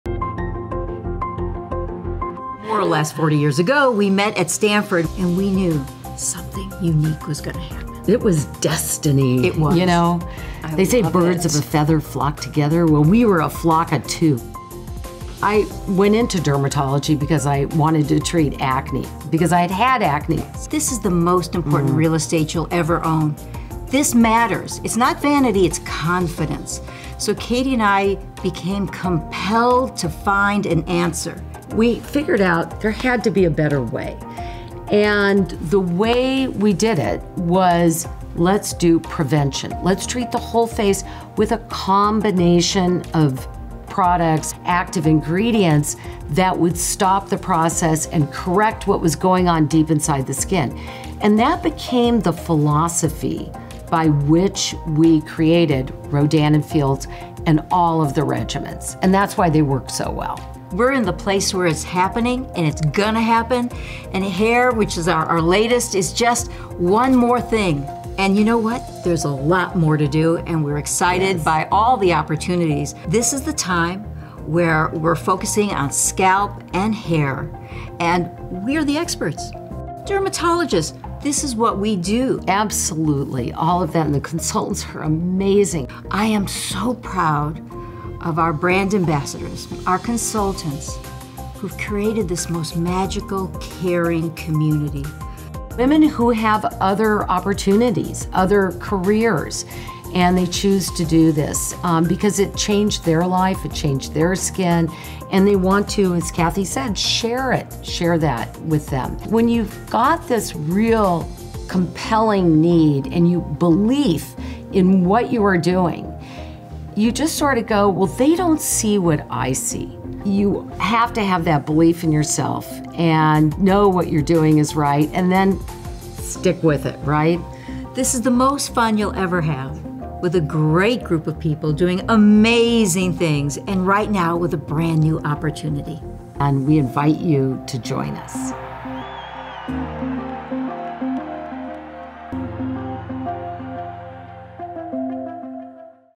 Meet Dr Katie Rodan and Dr Kathy Fields
Hear the story of how it all began from the Founders themselves and why now is the time to get started with Rodan + Fields.